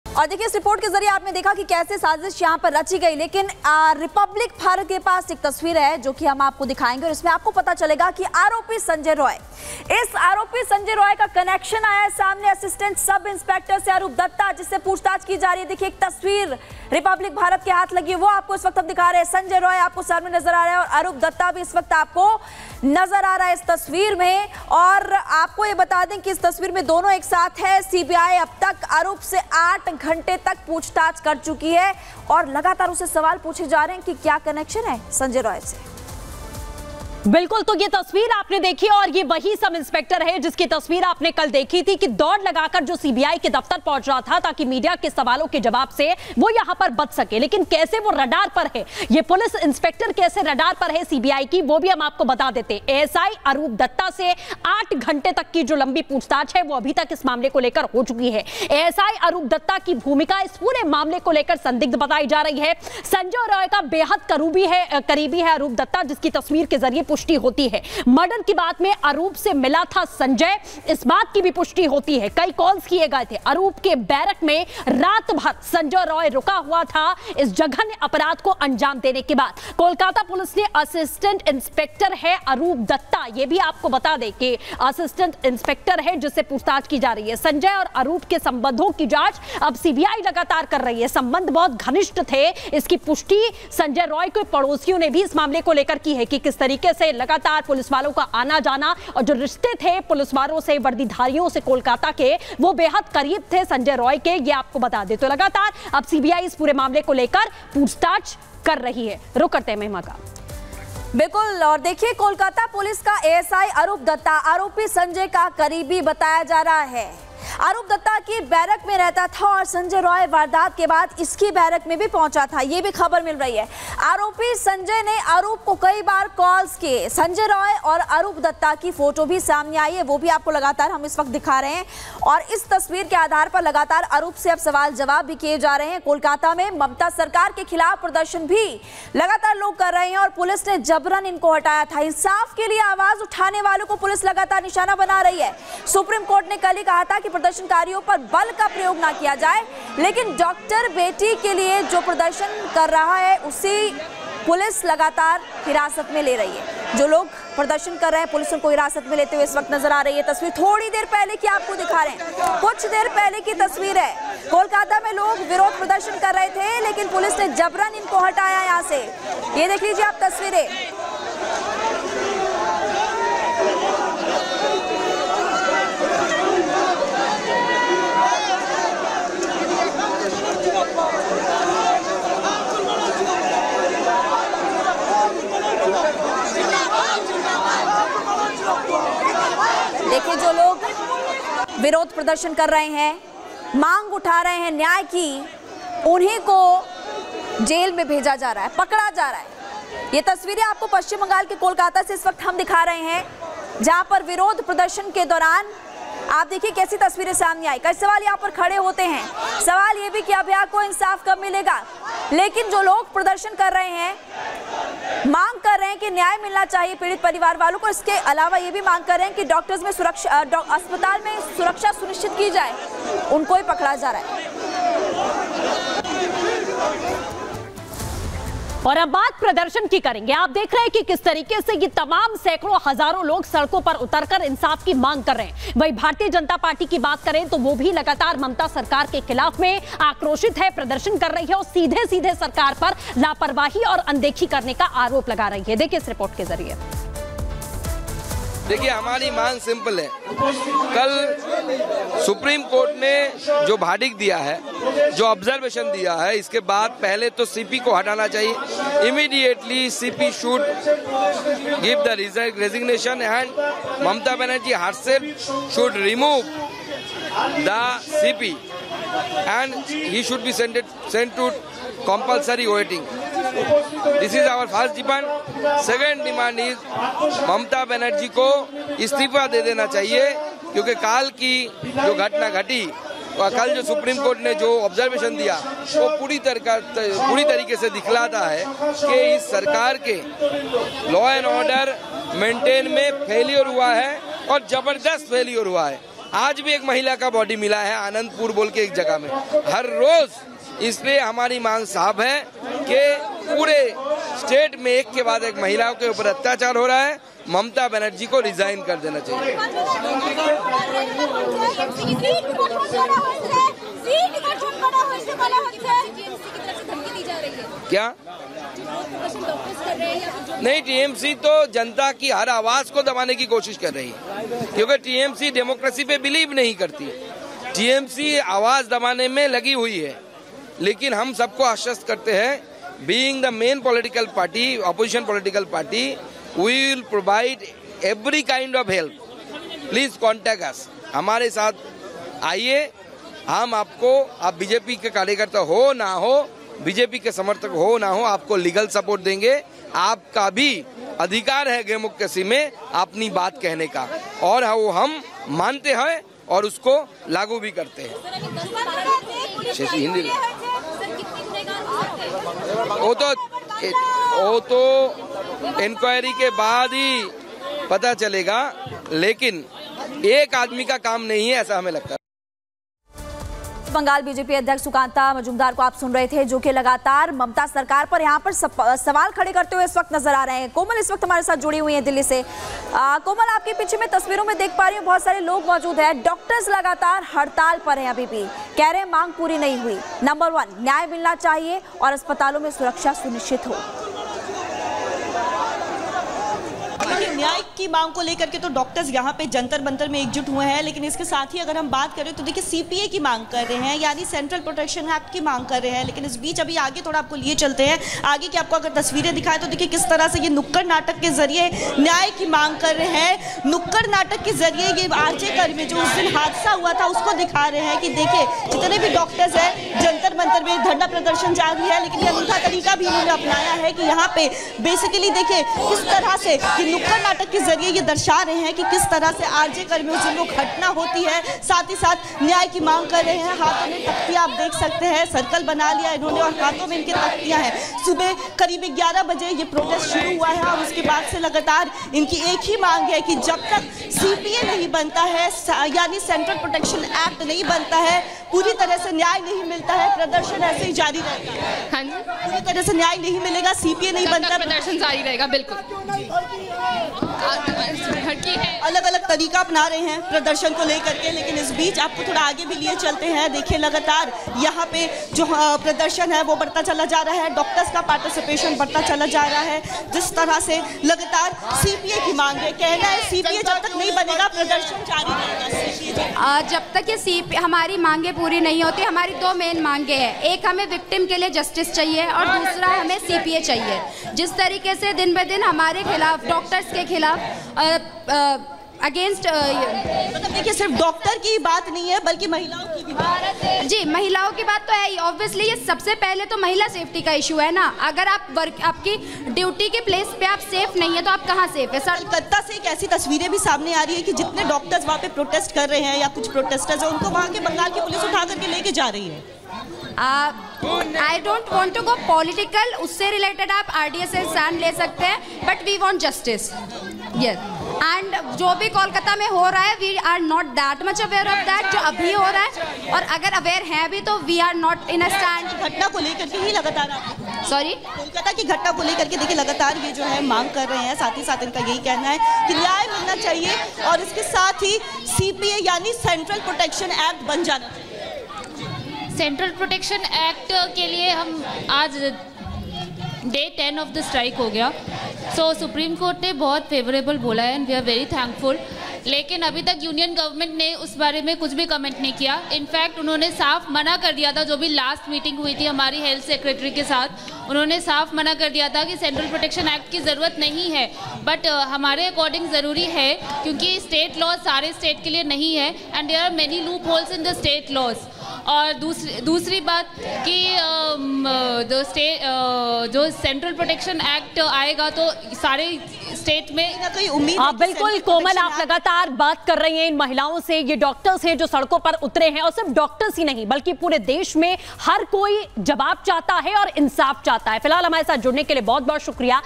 Kolkata Rape Murder Case: CBI की रडार पर ASI अरूप दत्ता, आरोपी संजय रॉय का है करीबी | R Bharat
कोलकाता में महिला जूनियर डॉक्टर के साथ दुष्कर्म और हत्या के विरोध में डॉक्टरों की हड़ताल दसवें दिन भी जारी है। इससे दिल्ली में स्वास्थ्य सेवाएं बाधित हो रही हैं और मरीज प्रभावित हो रहे हैं। हड़ताल से दिल्ली में अब तक 10 हजार से अधिक सर्जरी टल गई। साथ ही वेटिंग लिस्ट लंबी हो गई है। अकेले एम्स में अब तक पांच हजार मरीजों की सर्जरी टल गई है।